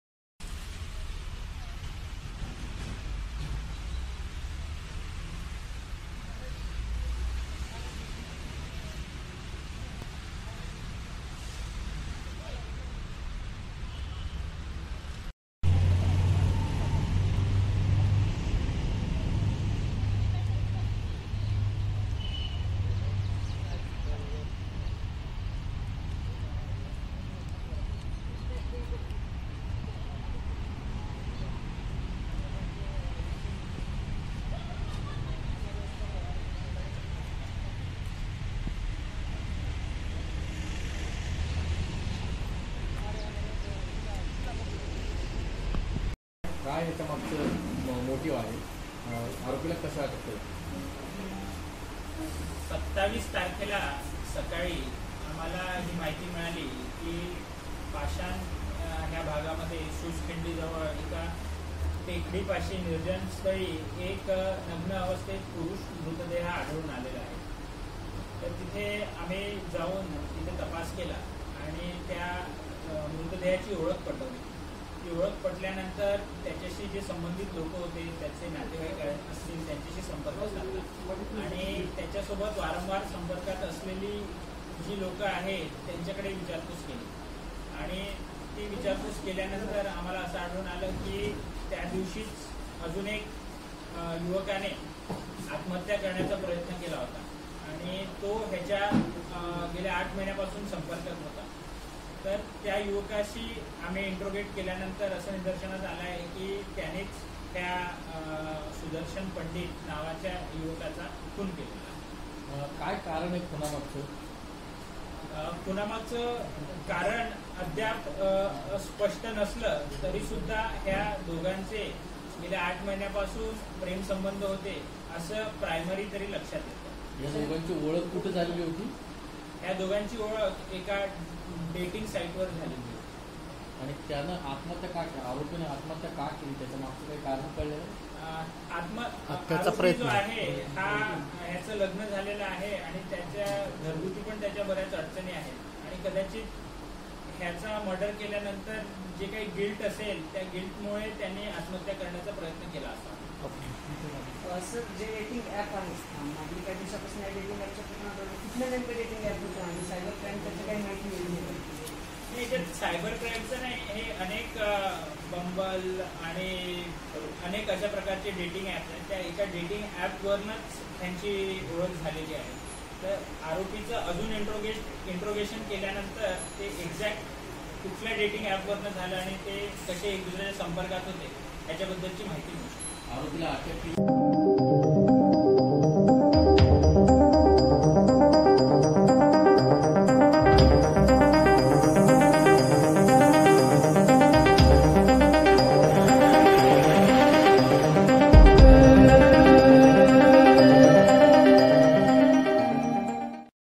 चतुःशृंगी पोलिसांनी विद्यार्थ्यांच्या खूनाचे गूढ उकलले
राष्ट्रीय रासायनिक प्रयोगशाळेत (एनसीएल) पीएचडी करणाऱ्या तरुणाच्या खूनाचे गूढ उकलण्यात चतुःशृंगी पोलिसांना यश आले आहे. प्रेमप्रकरणातून एकाने विद्यार्थ्यांचा गळा चिरून चेहरा दगडाने ठेचत खून केल्याचे उघडकीस आले आहे. त्यानंतर त्याचा मृतदेह सुस खिंडीत टाकला होता. याप्रकरणी रविराज क्षीरसागर (वय 35) याला पोलिसांनी अटक केली आहे. सुदर्शन उर्फ बाल्या बाबुराव पंडीत (वय 30, रा. शिवनगर, सुतारवाडी, मूळ रा. जानेपळ,जि. जालना) असे खून झालेल्याचे नाव आहे.